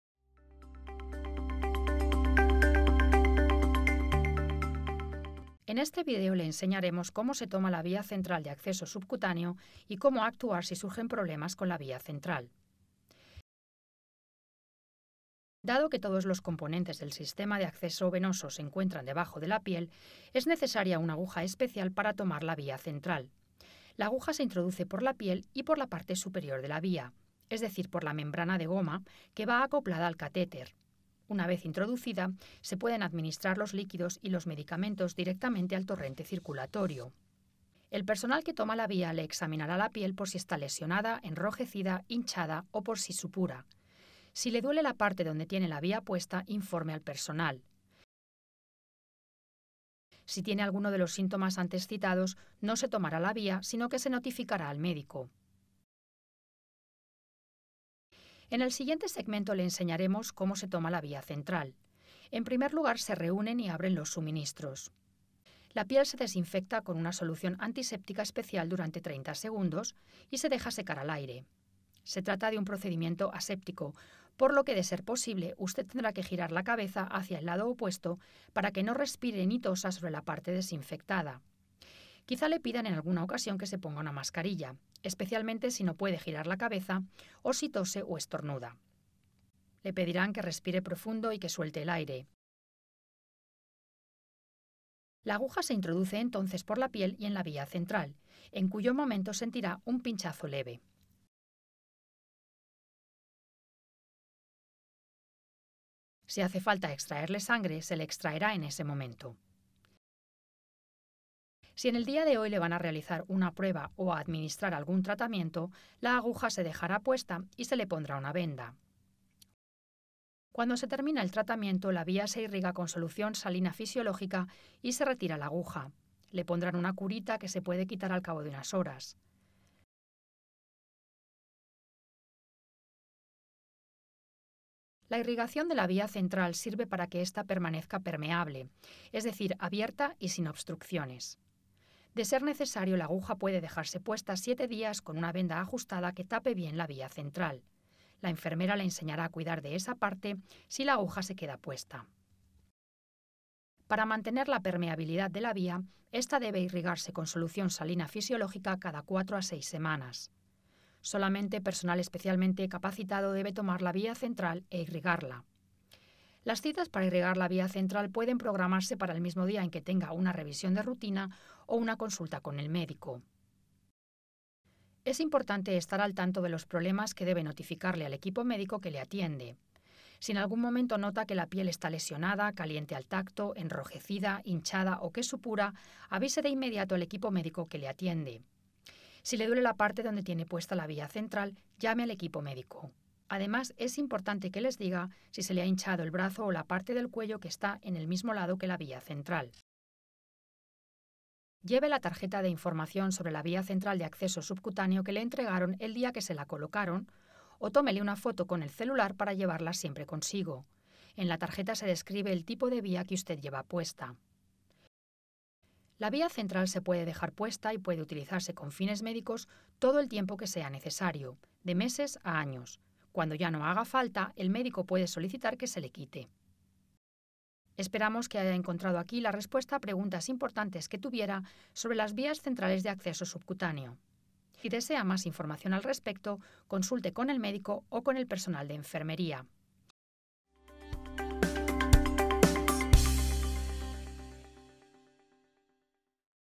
Vías centrales: toma y cuidado
En este video se muestra cómo se toma una vía central y qué medidas tomar si surgen problemas